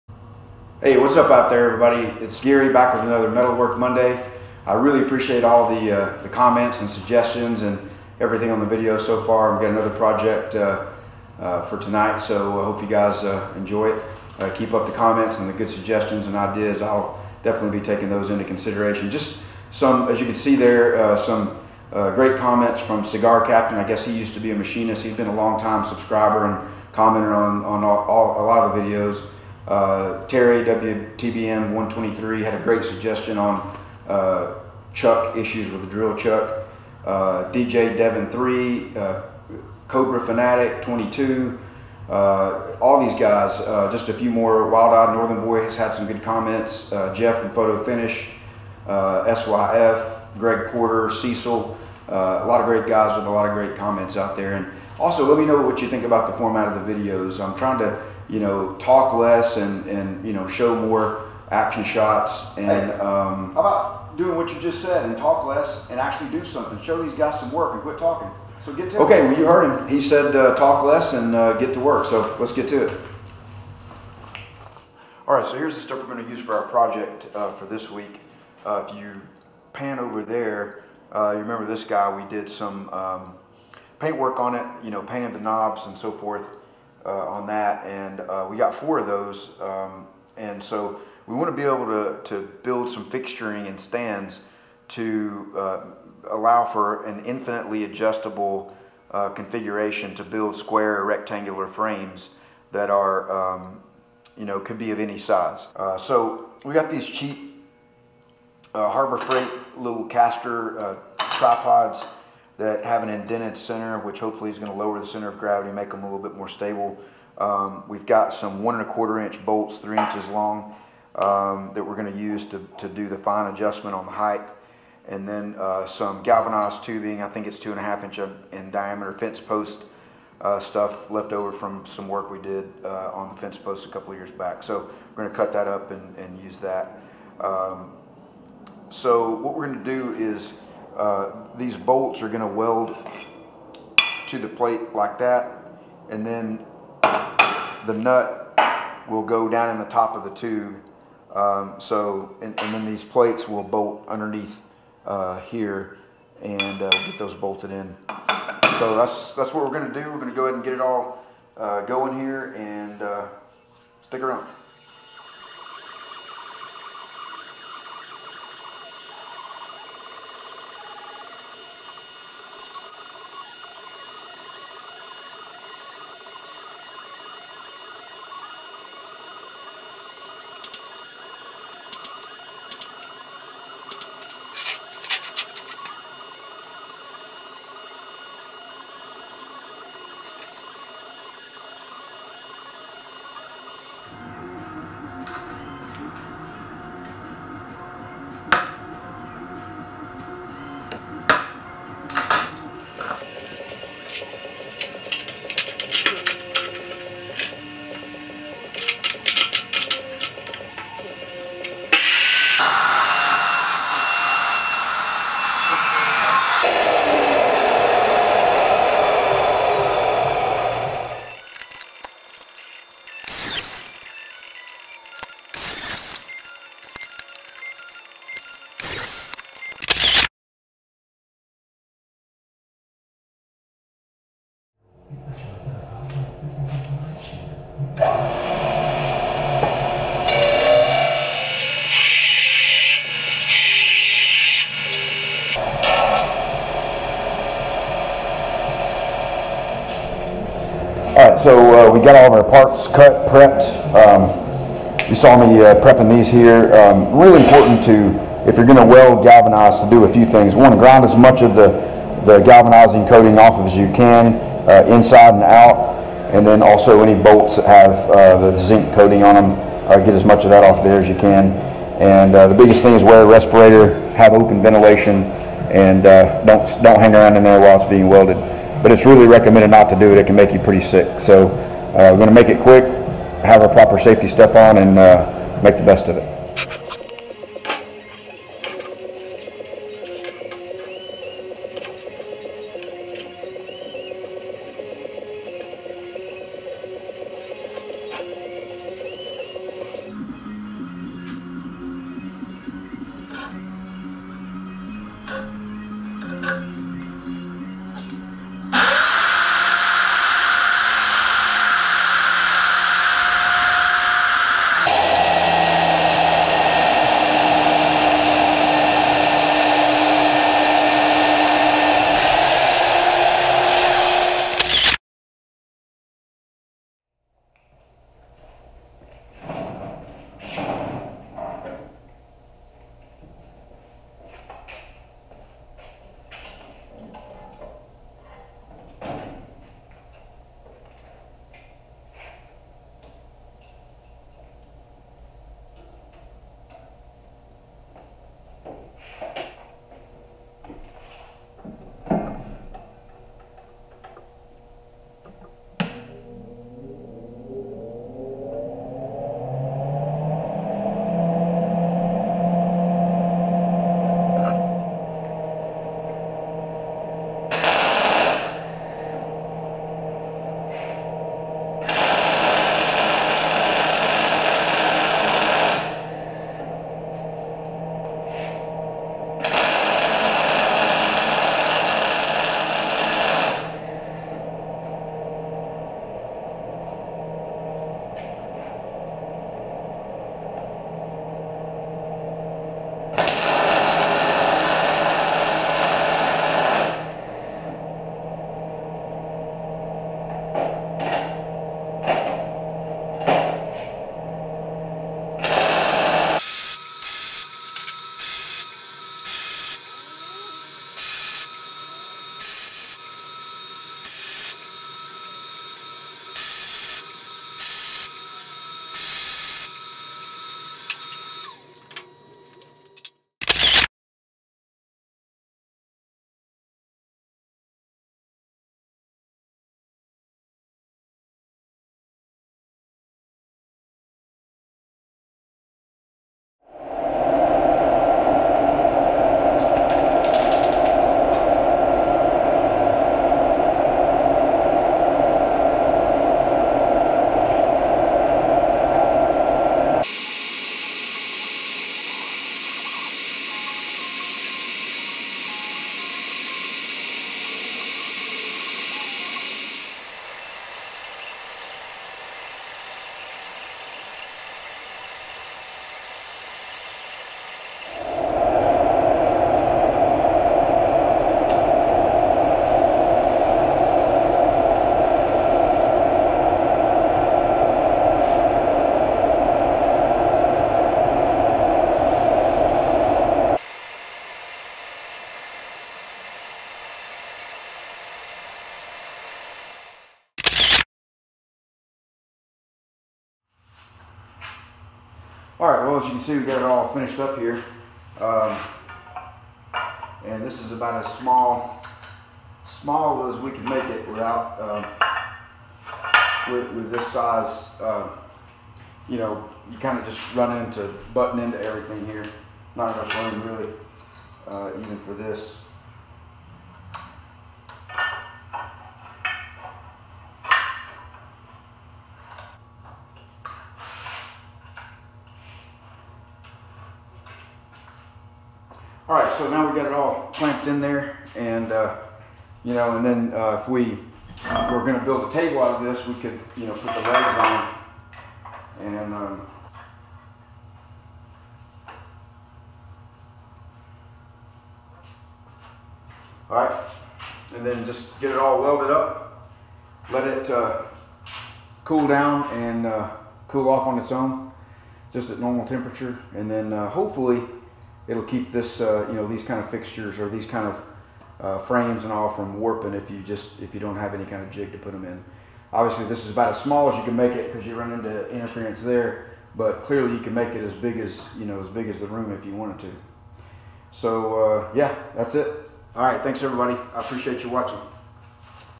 Metalwork Monday 4 - Stronghand Tools 3 Axis Vise/Vice Fixture Fabrication
In this Sunday episode of Metalwork Monday we are building some stands/mounts for our Stronghand Tools 3 Axis Vise Vices.  We did some cutting, grinding, and mig welding, as well as painting using Urekem base and clear.

Featured in this video are the clamps or vise you can use to clamp corners, edges, etc while you weld them.  3 legged holder is what they should be called because you can hold 3 legs with them.

Welding corners is one of the hardest things to do to keep straight and from warping.  These clamps are perfect for that.  Anyone that does Metalworking or Metal fabrication could use these type of clamps. They are made by stronghand tools.  These clamps can be used for Mig Welding, Tig welding, brazing, etc.